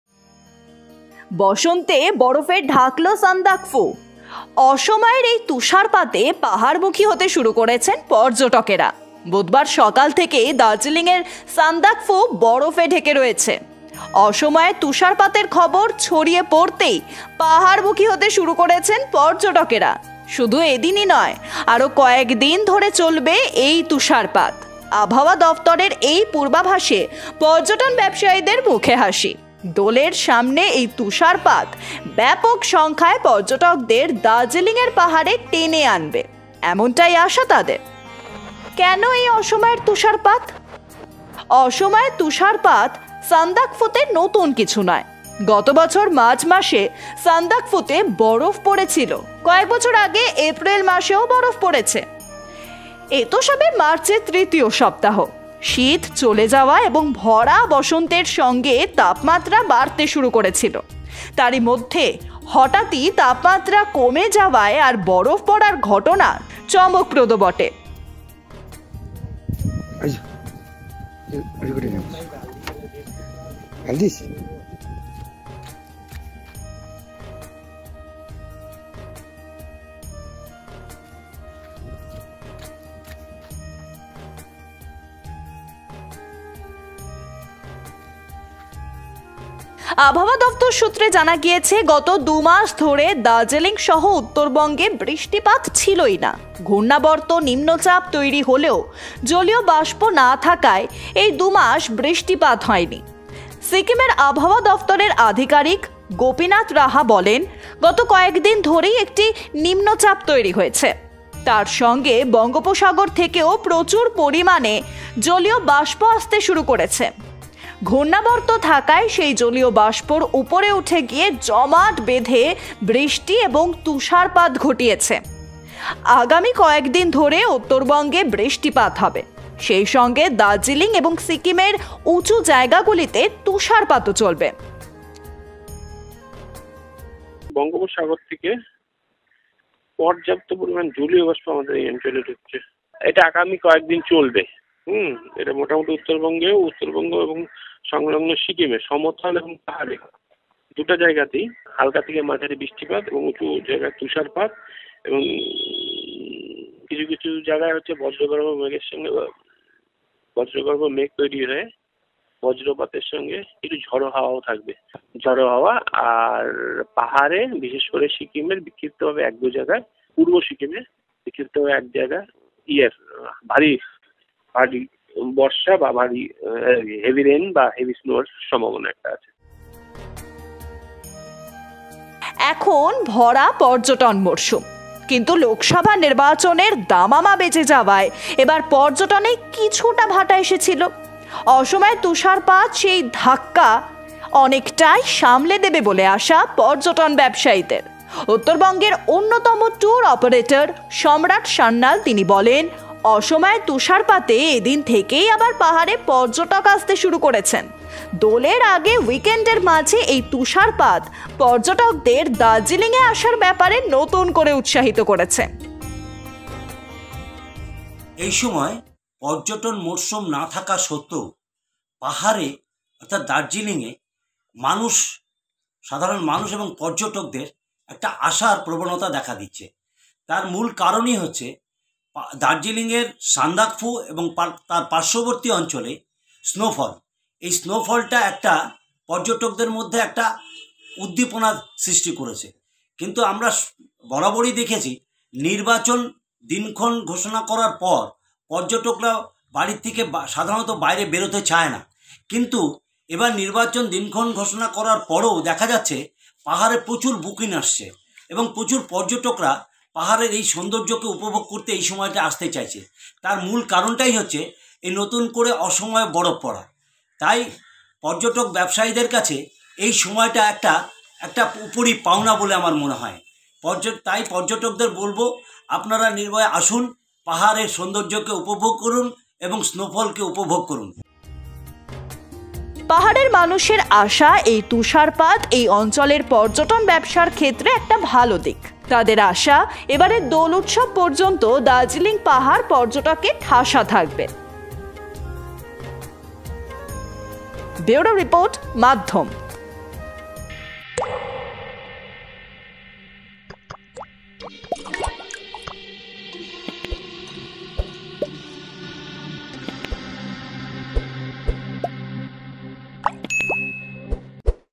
Sandakphu Snowfall: বসন্তে বরফে ঢাকলো সান্দাকফু | Bangla News
Sandakphu snowfall: Sandakphu covered with snow in spring

বসন্তে বরফে ঢাকলো সান্দাকফু।
অসময়ের এই তুষারপাতে পাহাড়মুখী হতে শুরু করেছেন পর্যটকেরা।
বুধবার সকাল থেকেই দার্জিলিংয়ের সান্দাকফু বরফে ঢেকে রয়েছে। অসময়ের তুষারপাতের খবর ছড়িয়ে পড়তেই পাহাড়মুখি হতে শুরু করেছেন পর্যটকরা।  শুধু এদিনই নয়, আরও কয়েকদিন ধরে চলবে এই তুষারপাত।